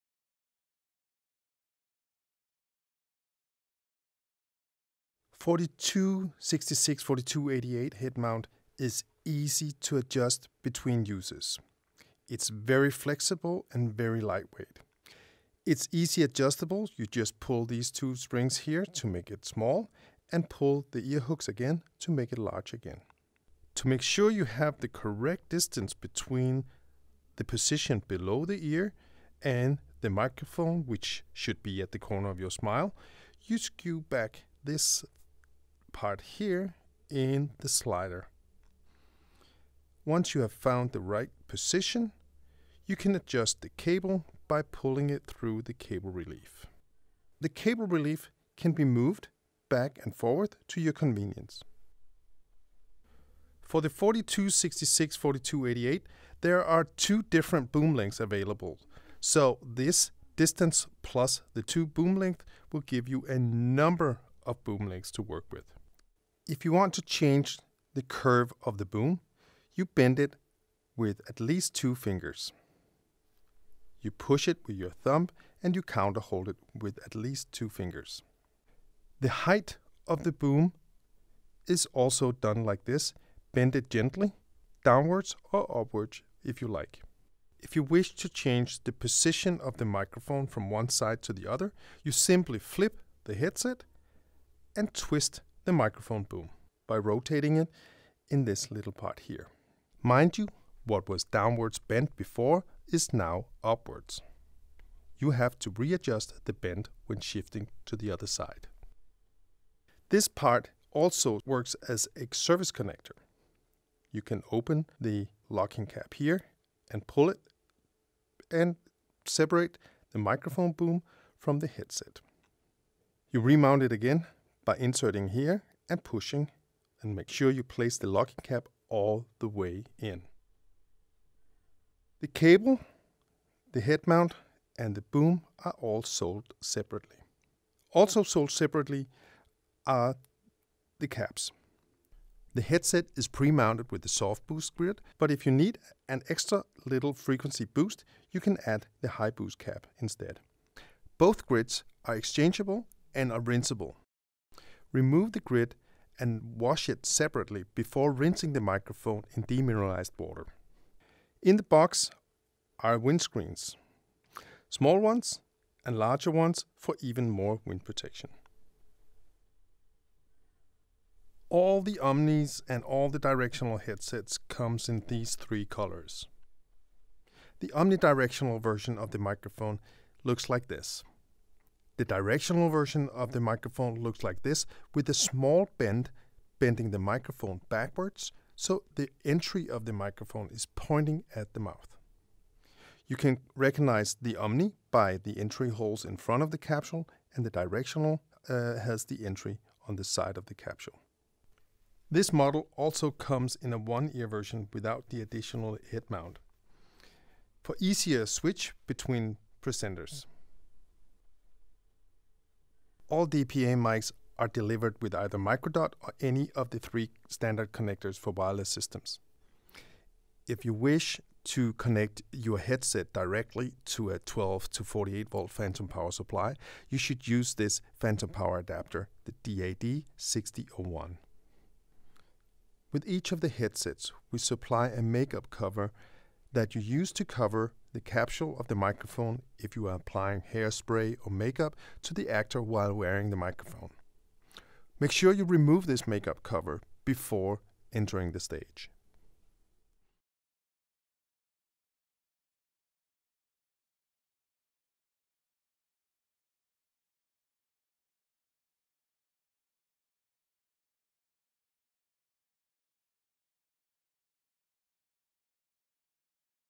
Learn how to mount and adjust 4266/4288 Headset Microphones properly
Mic placement is important, particularly with directional mics. This video walks you through how to adjust the 4266 Omni Headset Mic and the 4288 Directional Headset Mic so that it fits snuggly, comfortably and properly on the head. You will learn how to position the boom as well as adjust the headset frame and the ear hooks.

Chapters
0:00 Mic placement
0:14 Adjusting headset frame and boom and cable
1:06 Adjusting boom curve
1:28 Flipping the boom
2:35 Cleaning the microphone
2:51 Variants
3:31 Connectors & adapters